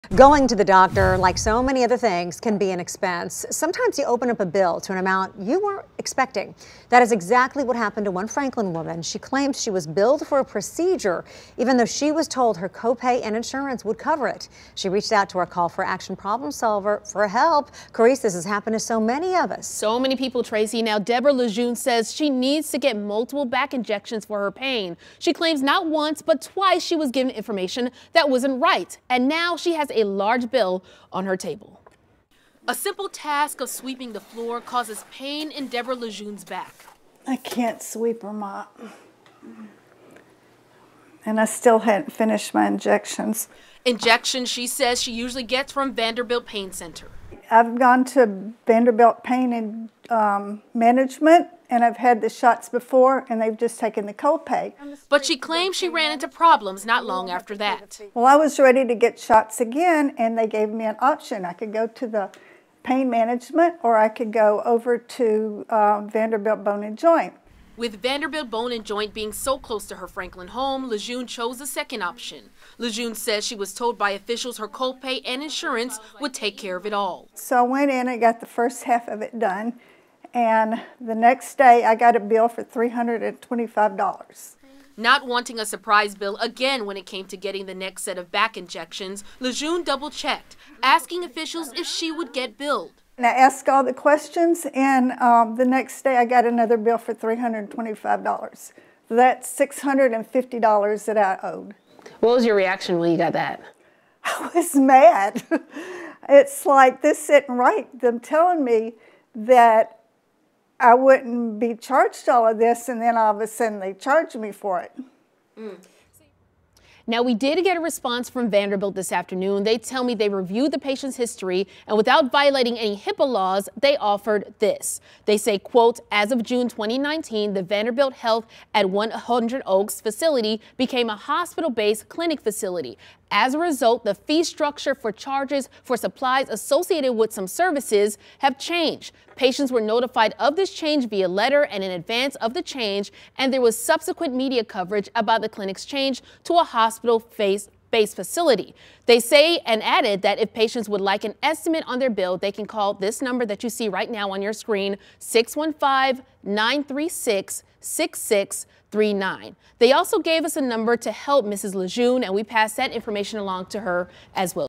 Patient with back pain gets unexpected medical bill, looks for answers
A simple task of sweeping the floor causes pain in Deborah LeJeune’s back.